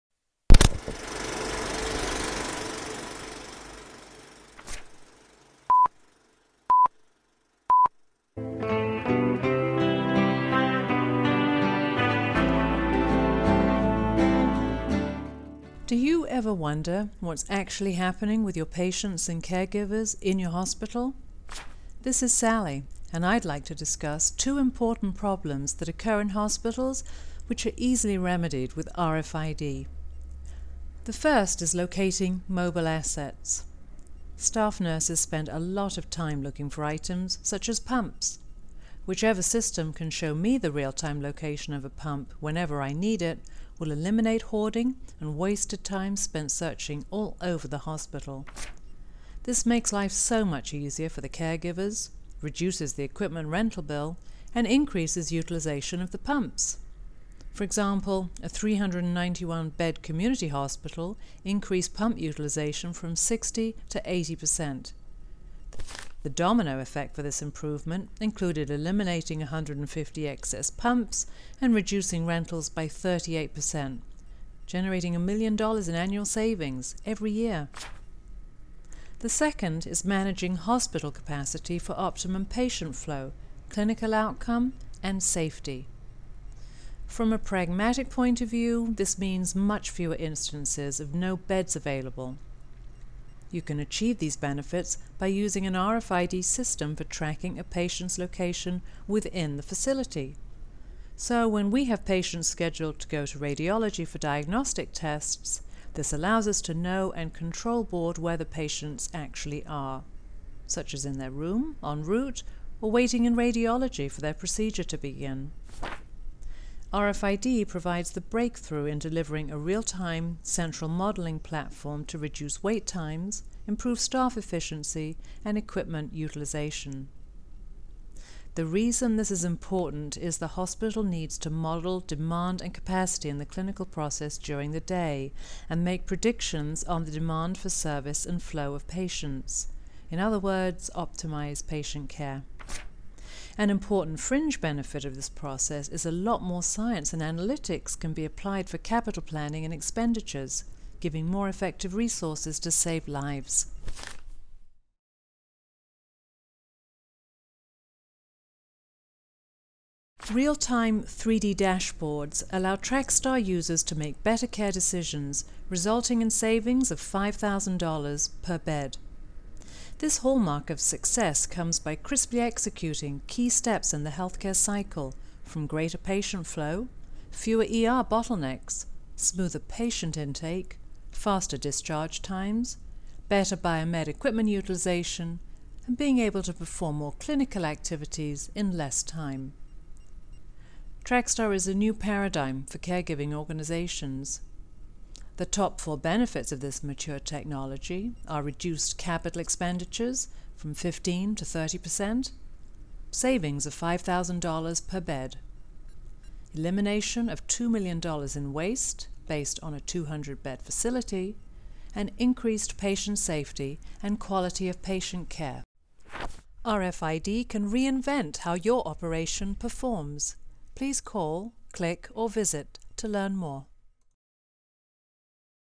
RFID Healthcare Movie
A short by American RFID Solutions showing 2 possible scenarios in a hospital and explaining how RFID system can resolve these situations in the healthcare section.

American RFID Solutions
Visit 1522 East Algonquin Road, Arlington Heights, IL 60005